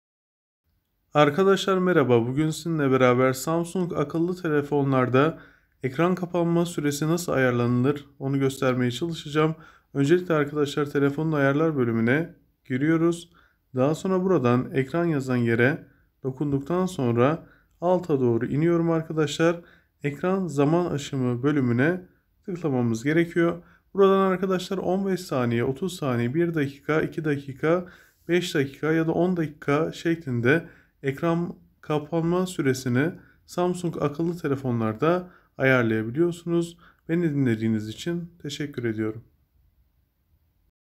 Samsung Ekran Kapanma Süresi Nasıl Ayarlanır
Samsung akıllı telefonlarda ekran kapanma süresi ayarlama hakkında bilgi vermeye çalıştık.

Samsung telefonlarında ekran kapanma süresi yani ekran zaman aşımı ayarı, belirli bir süre boyunca cihazın kullanılmaması durumunda ekranın otomatik olarak kapanmasını sağlar. Bu ayar, pil ömrünü korumak ve ekranın gereksiz yere açık kalmasını önlemek için kullanışlıdır. Aşağıda Samsung telefonlarında ekran kapanma süresini nasıl ayarlayabileceğinizle ilgili adımlar için videomuzu izleyebilir ve gerekli adımları uygulayarak ekran kapanma süresini ayarlayabilirsiniz.

Samsung telefonlarda ekran zaman aşımı nasıl değiştirilir hakkında bilgi vermeye çalıştık. Umarım yardımcı olabilmişimdir.